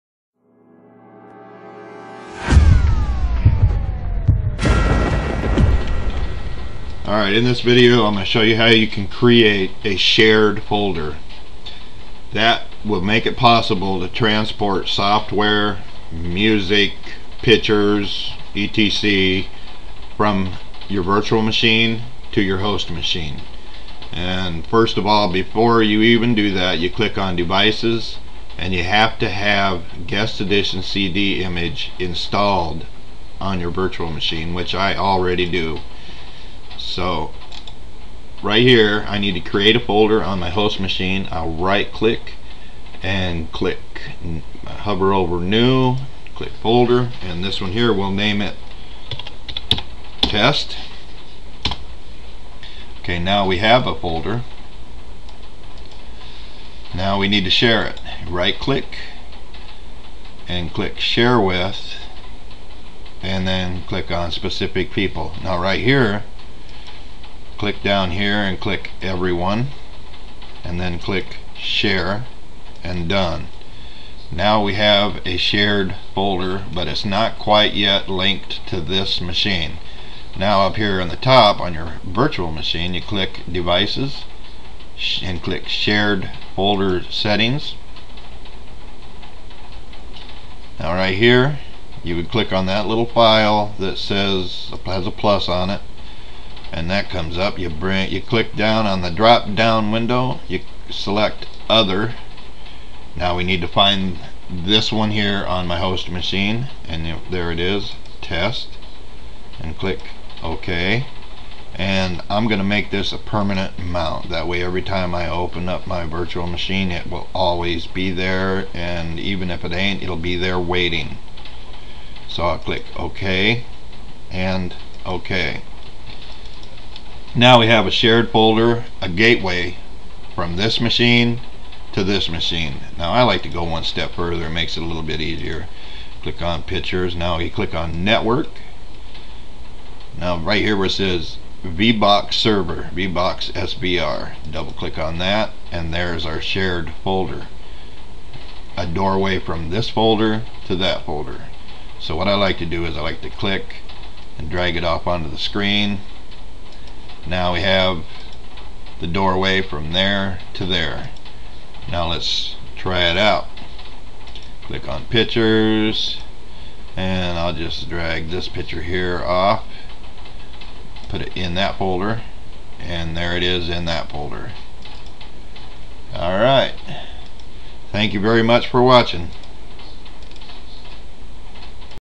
How to create a shared folder for Oracle VirtualBox By Lenny Parker
With a shared folder you can transport Temp files,Music,Pictures and Software between your Host Machine and your Virtual Machine Very easily.